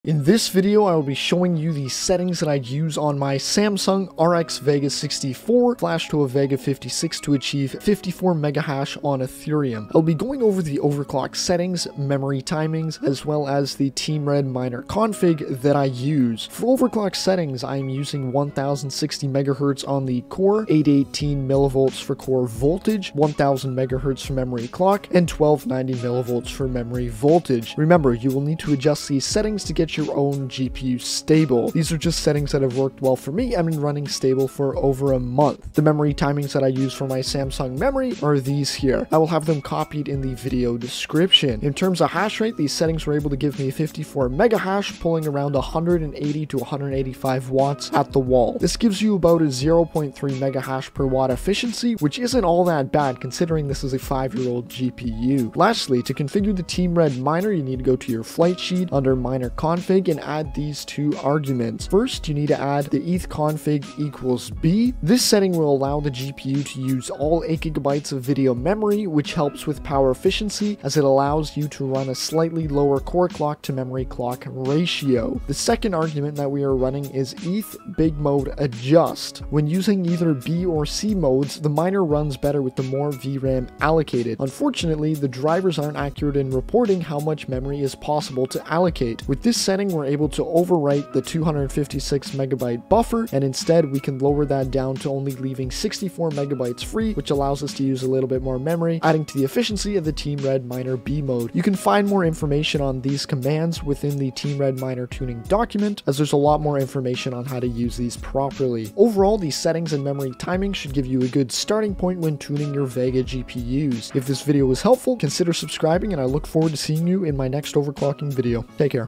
VEGA 64/56 Ethereum Overclock Memory Timings 54Mhs+
In this video I go over the Overclock Settings I used while mining Ethereum on the RX VEGA 64 (Flashed to Vega 56 vbios).

The Overclock Settings I used:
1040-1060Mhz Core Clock
818mv Core Voltage (you may need to run up to 850mv)
1000Mhz Memory Clock (you may need to run 960 up to 1040Mhz)
1290Mv Memory Voltage (you may need to run higher upto stock)

These Settings along with these Samsung memory Timings got me 54Mhs using the TEAM RED MINER, B mode.

Team Red Miner config:
--eth_config=B440
--eth_big_mode_adjust=64

COPY AND PASTE Samsung Memory Timings:
amdmemtweak --CL 20 --RC 37 --RP 11 --WR 14 --CWL 8 --FAW 12 --RAS 20 --REF 65535 --RFC 248 --RTP 5 --RRDL 6 --RRDS 3 --WTRL 9 --WTRS 4 --RCDRD 12 --RCDWR 12

These should work for both Vega 56 and Vega 64.
My GPU is a VEGA 64.

Time stamps:
0:00 Intro
0:16 Overclock settings
0:35 Memory Timings
1:00 Team Red Miner Config IMPORTANT SETTINGS